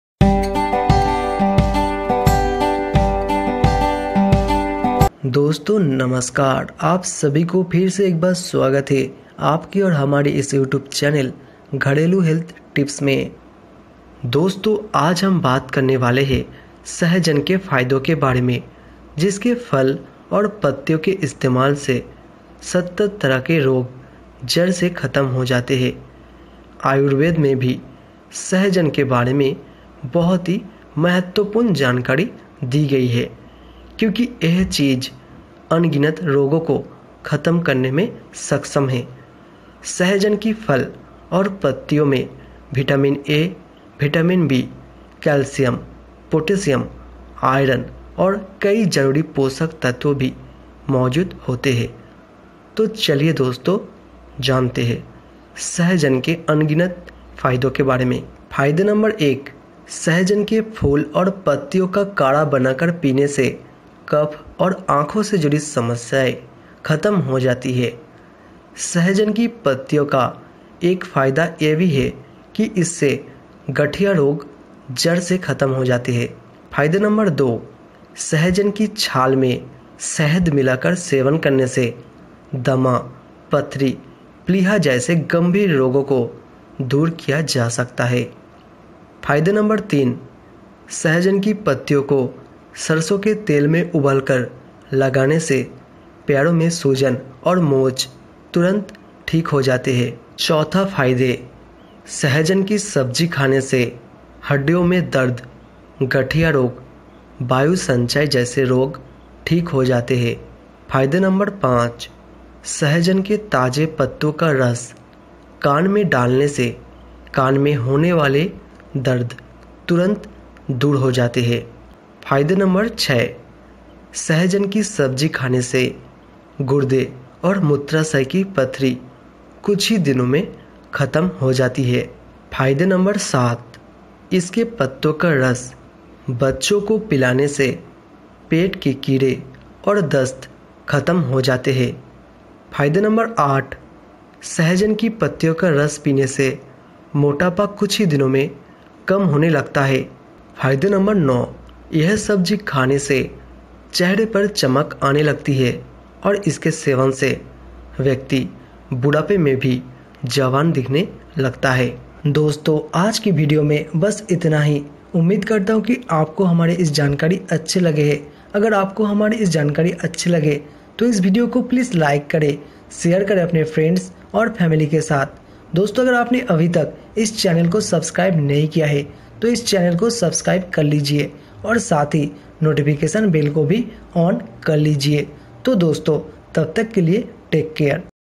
सहजन के चमत्कारी फायदे || 70 रोगों को जड़ से ख़तम करता है ये संजीवनीबुटी || Benefits Of Drumsticks
Some Health Benefits Of Drumsticks / Moringa

1) Drumsticks For Eyes Problems
2) Drumsticks for Skin problem
3) Drumsticks for kidney stone
4) Drumsticks For Arthritis
5) Drumsticks For Weight Loss